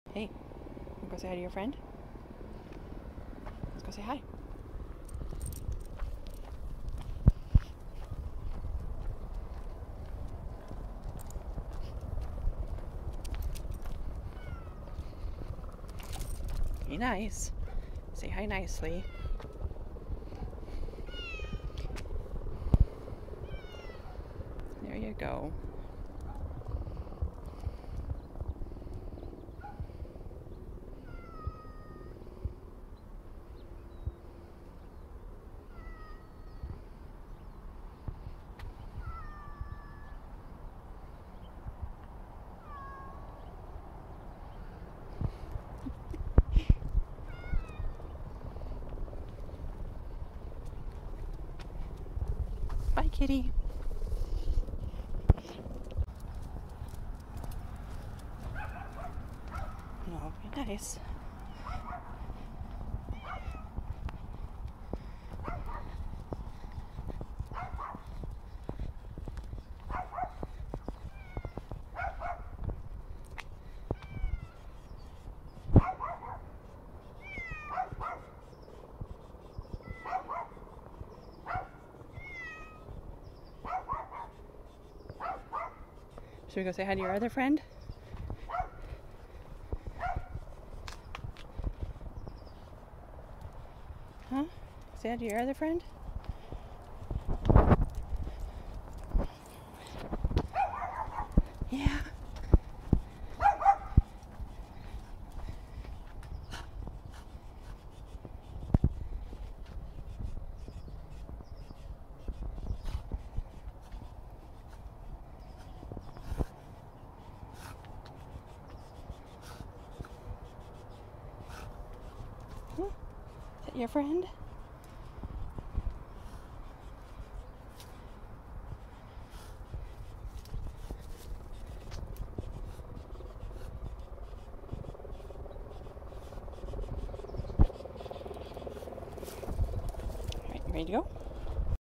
Sheltie gets to see her friends! A very chatty cat
These are my dogs 2 "out on a walk" friends.

ps sorry for the sound there was a helicopter flying over head & I don't have a mic cover yet to block air sound.